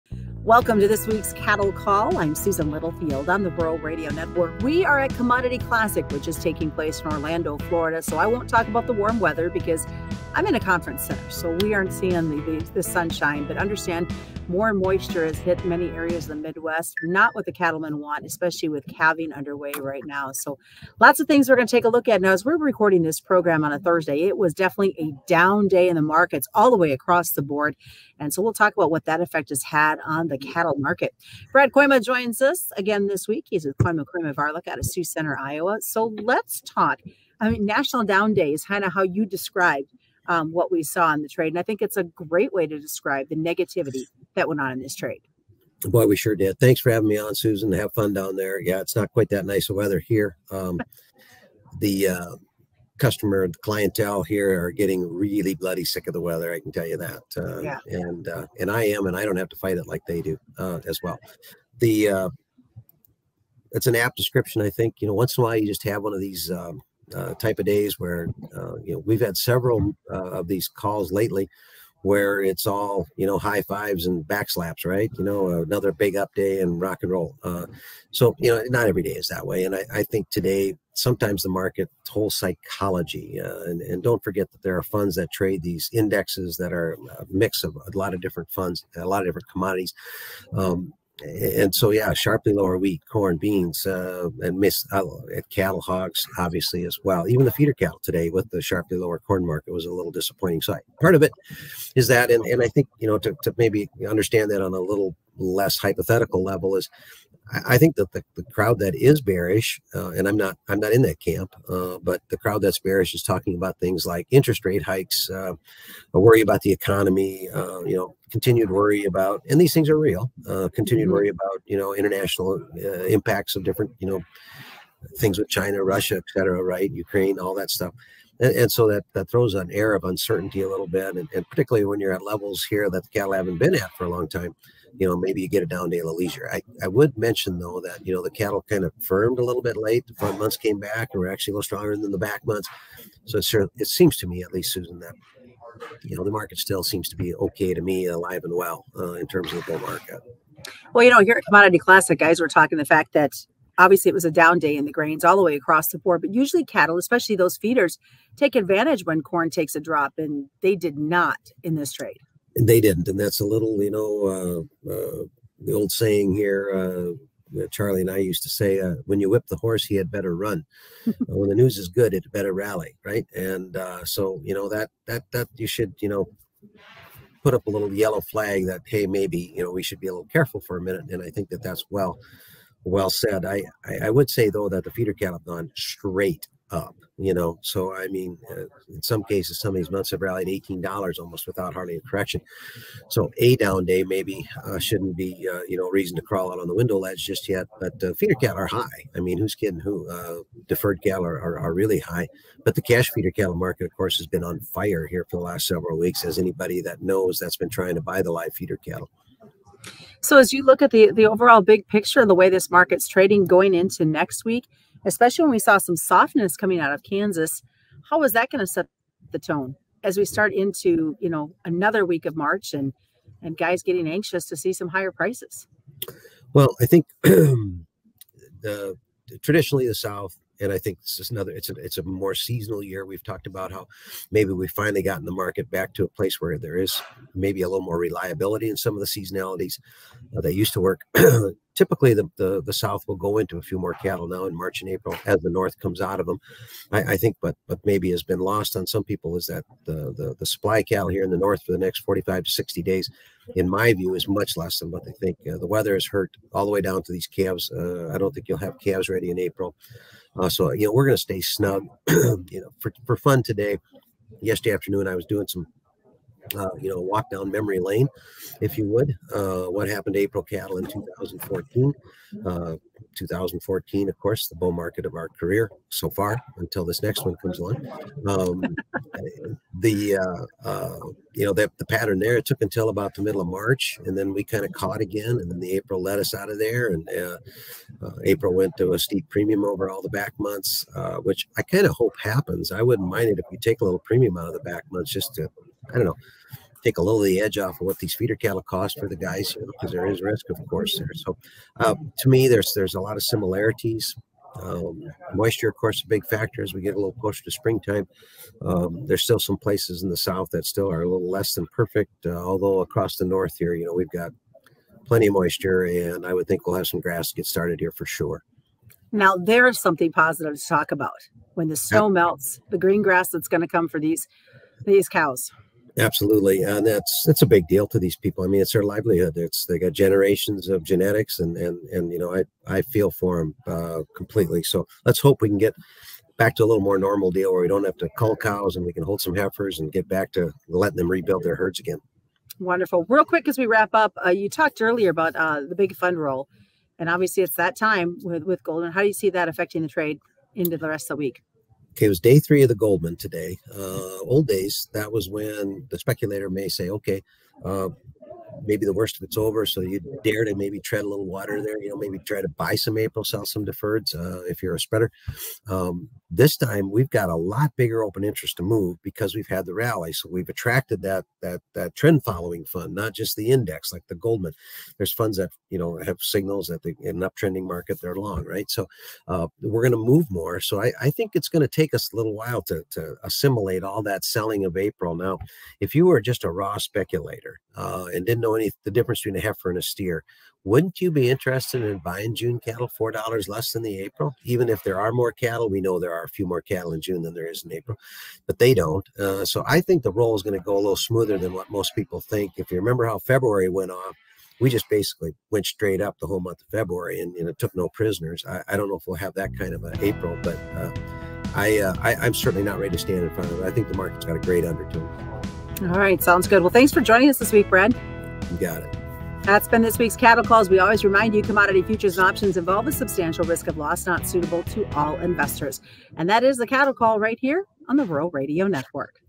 Lower feeders this week 3 10 23 Cattle Call
Cattle markets had another interesting ride this week.  Brad Kooima with Kooima Kooima Varilek said the weather is also taking a toll on the cattle producer.  Thursday brought negativity to the markets and that influenced the rest of the week of trading & maybe setting the movement for Monday.